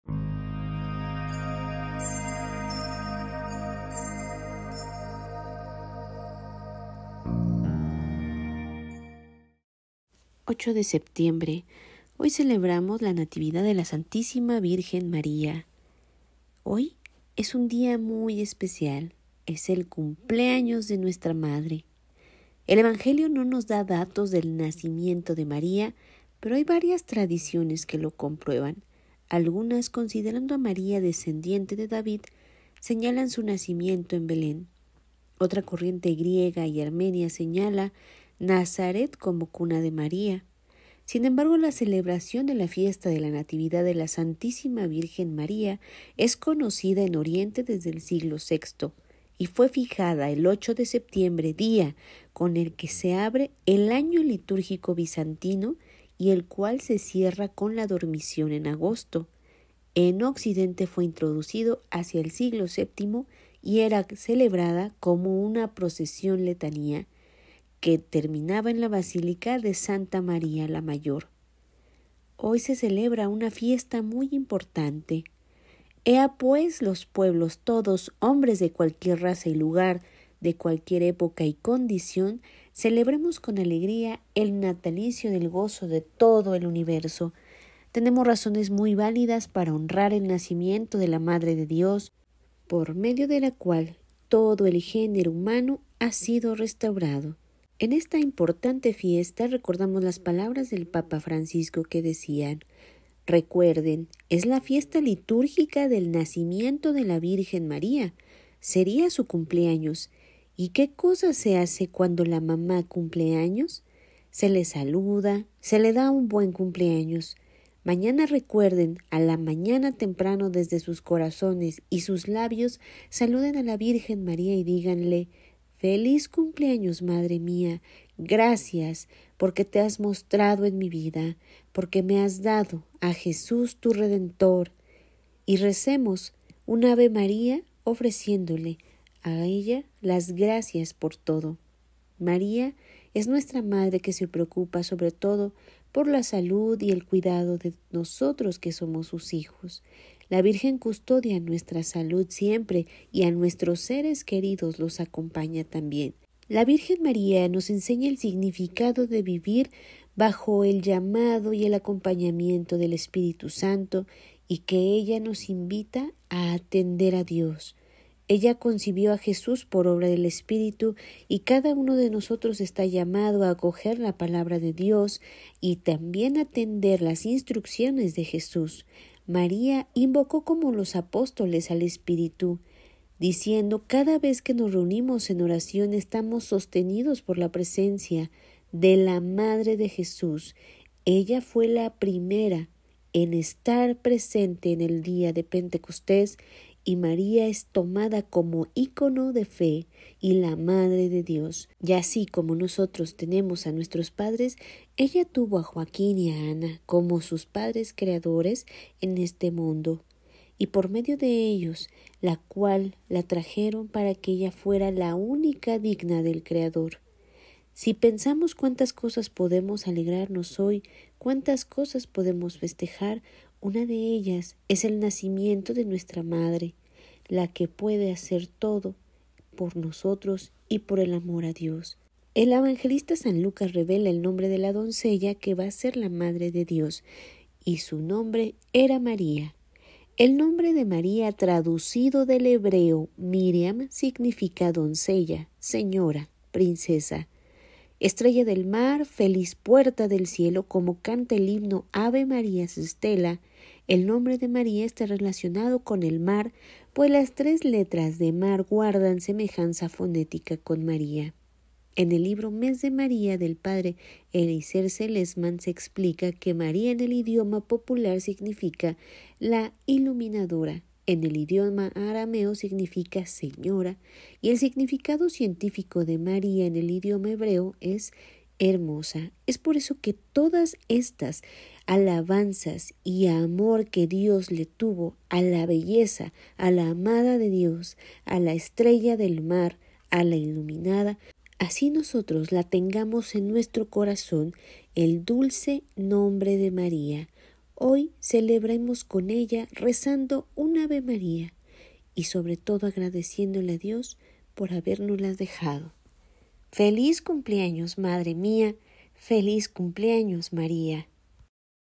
Fiesta de la natividad de la Virgen María
Hoy celebremos con ella rezando un ave María y sobre todo agradeciéndole a Dios por haberla dejado.
¡Feliz cumpleaños madre Mía!
¡Feliz cumpleaños María!